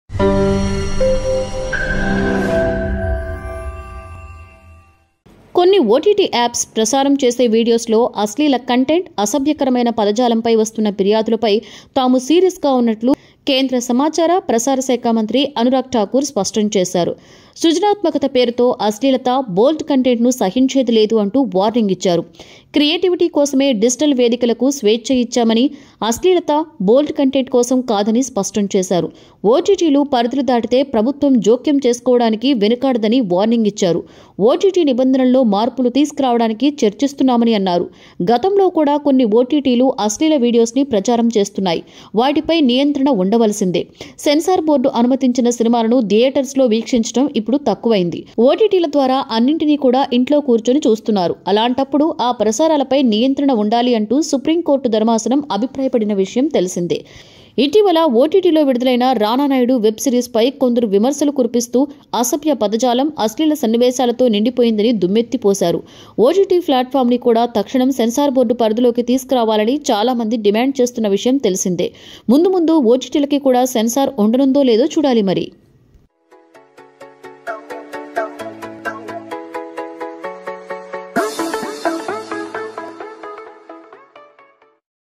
అడ‌ల్ట్ కంటెంట్‌పై కేంద్రం వార్నింగ్‌.. | OTT movies | Anurag thakur | sankartv
అడ‌ల్ట్ కంటెంట్‌పై కేంద్రం వార్నింగ్‌..